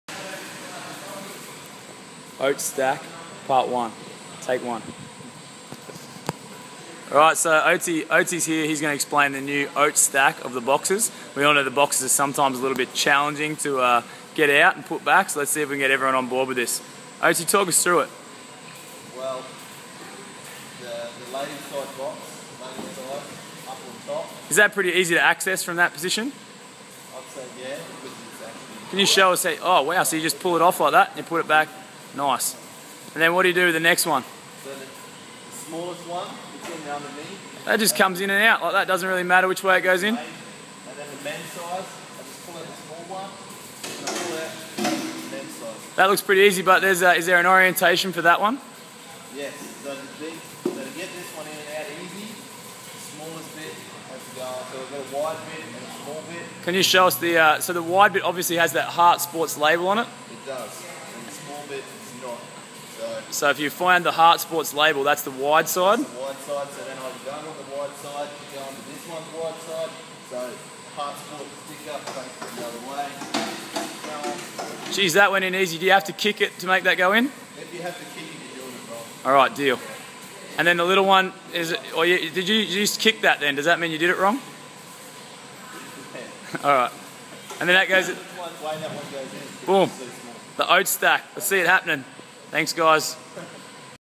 Oats stack
CrossFit Townsville plyo box stacking technique for both locations